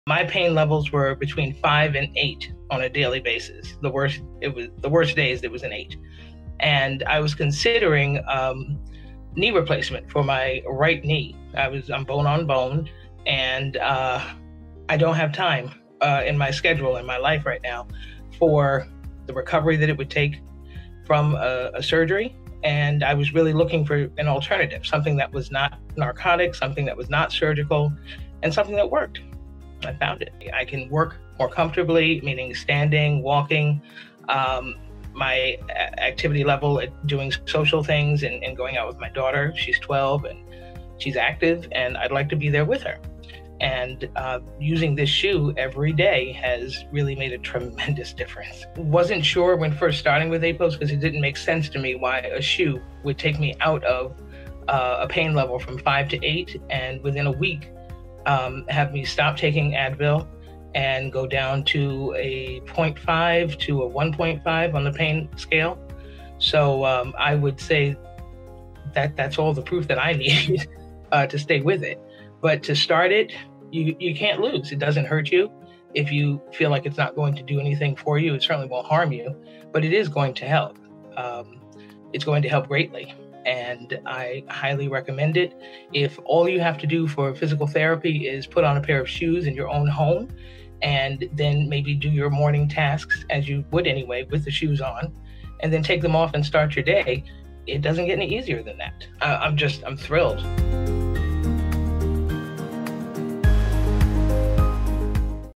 Margaret J. Testimonial
Margaret was contemplating surgery for her knee pain but did not have the time in her busy life to take off to recover. She was thrilled to find Apos®, a non-invasive treatment that as she says ‘WORKS!’.